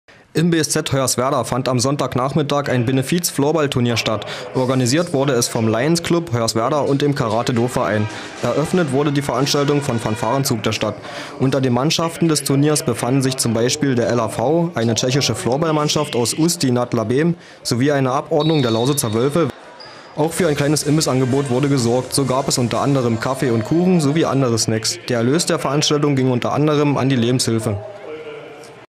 Benefiz-Tunier im BSZ
Im BSZ Hoyerswerda fand am Sonntagnachmittag ein Benefiz-Floorballtunier statt. Organisiert wurde es vom Lions Club Hoyerswerda und dem Karate Do Verein. Eröffnet wurde die Veranstaltung vom Fanfarenzug der Stadt.
Unter den Mannschaften des Tuniers befanden sich z.B. der LHV, eine tschechische Floorball-Mannschaft aus Usti Nad Labem, sowie eine Abordnung der Lausitzer Wölfe, welche auch den
Sieg holten. Auch für ein kleines Imbissangebot wurde gesorgt, so gab es u.a. Kaffee & Kuchen sowie andere Snacks.
Der Erlös der Veranstaltung ging unter anderem an die Lebenshilfe.